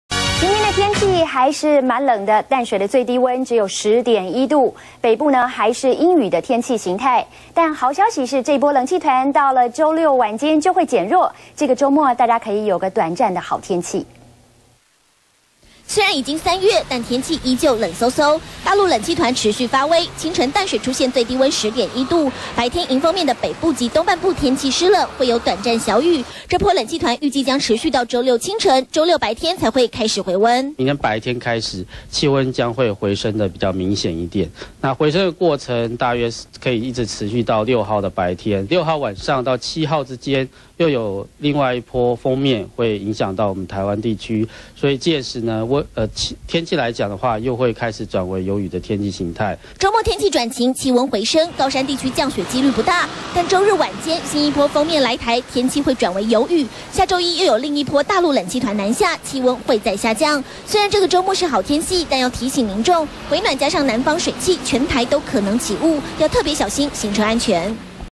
今晨低溫淡水10.1度 明天將回暖
中視新聞》好冷，4日清晨最低溫為淡水攝氏10.1度。中央氣象局表示，這波冷氣團最冷的時候已經過去，5日白天冷氣團減弱，各地氣溫可望回升到20度以上，不過，因冷氣團勢力衰退，南方水氣北移，西半部及離島局部地區不排除再次發生濃霧籠罩情形。